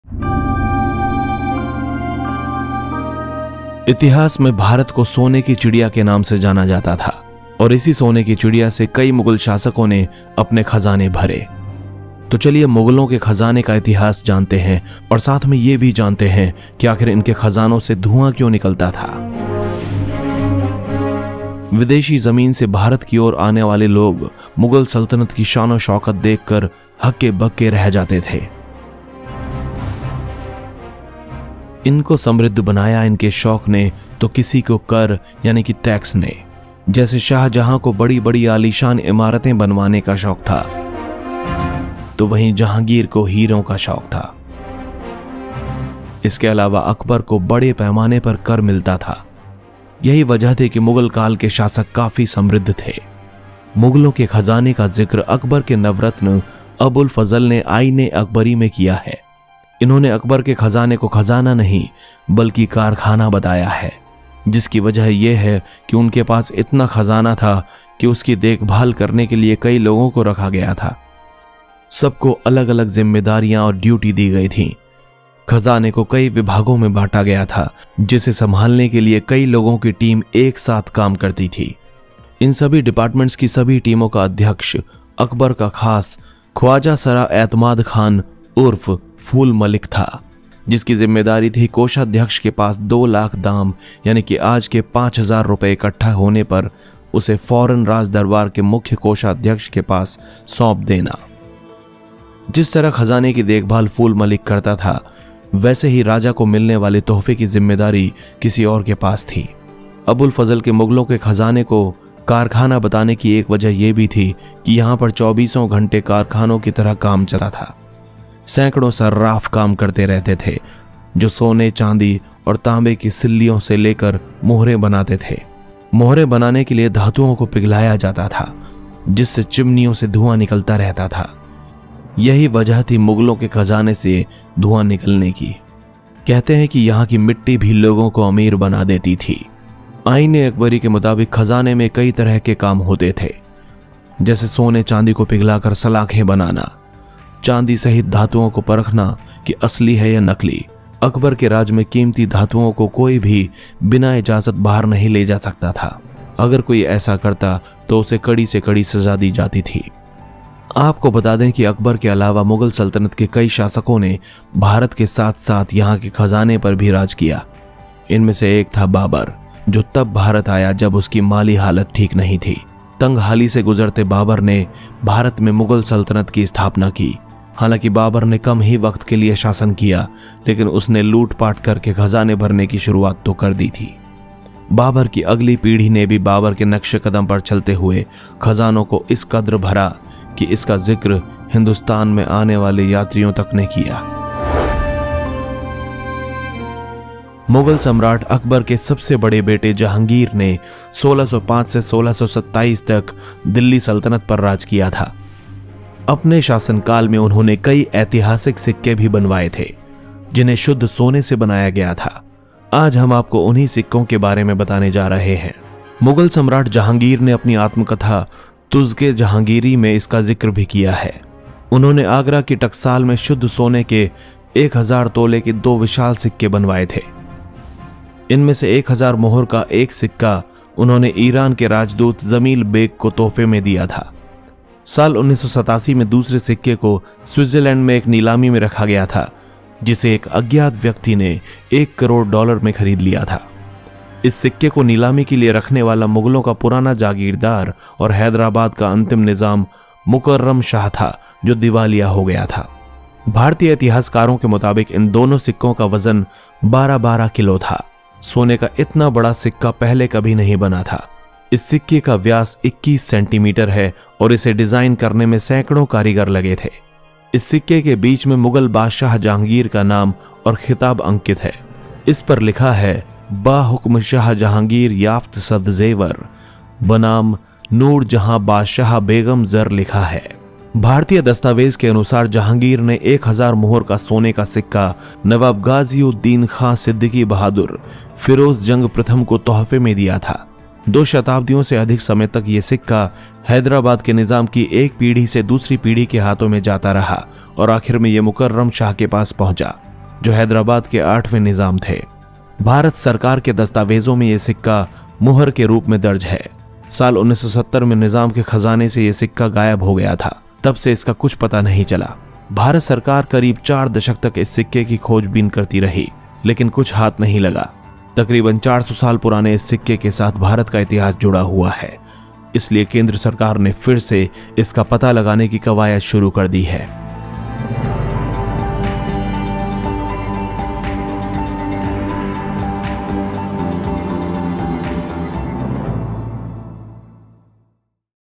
मुग़लों के अनजाने रहस्य! Mughal's Unknown Secrets | Mughal Treasure Mystery & BIGGEST Gold Coin
मुग़लों के ख़ज़ाने से धुआं क्यों निकलता रहता था? | Mughal Treasure Smoke Mystery & BIGGEST Gold Coin

सोने के 12 किलो के इस सिक्के से बड़ा सिक्का कभी बना ही नहीं था। इस सिक्के का व्यास 21 सेमी है और इसे डिजाइन करने में काफी कारीगर लगे थे। सिक्के के बीच में बादशाह का नाम और खिताब अंकित है। इस पर लिखा है ‘बा-हुक्म शाह जहांगीर याफ़्त सद ज़ेवर, बनाम नूरजहां बादशाह बेगम ज़र’।
वर्ष 1987 तक माना जाता रहा था कि ये सिक्का अब मौजूद नहीं है, लेकिन जब स्विटजरलैंड से नीलामी की खबर आई तो लोगों में उत्सुकता पैदा हो गई।
दस्तावेज के अनुसार एक हजार मूल्य का सोने का सिक्का जहांगीर ने नवाब गाजीउद्दीन खां सिद्दीकी बहादुर, फिरोज जंग प्रथम को तोहफे में दिया था। इनके पुत्र निजाम-उल-मुल्क ने आसफशाही राजवंश की स्थापना की थी। दो शताब्दियों से ज्यादा समय तक ये सिक्का निजाम की एक पीढ़ी से दूसरी पीढ़ी के हाथों में जाता रहा और आखिर में ये मुकर्रम शाह के पास पहुंच गया जो 8वें निजाम थे। सिक्के की नीलामी हुई और इसे एक अज्ञात व्यक्ति ने एक करोड़ डालर में खरीद लिया।